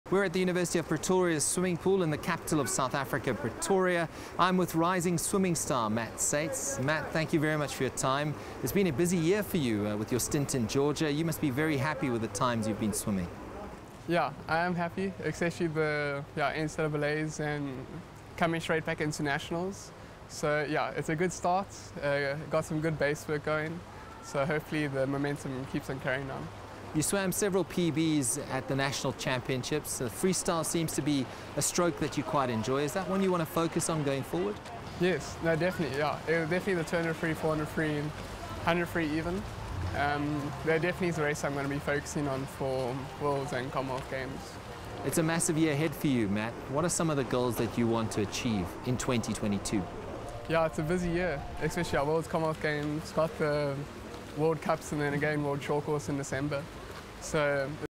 New generation of swimmers making waves in South Africa
There is a new generation of swimmers making big waves in South Africa and one of them is 18-year-old Matt Sates, who has just returned home from winning the coveted N-C-A-A 500 yard title at the University of Georgia and is already being compared to the legendary Michael Phelps.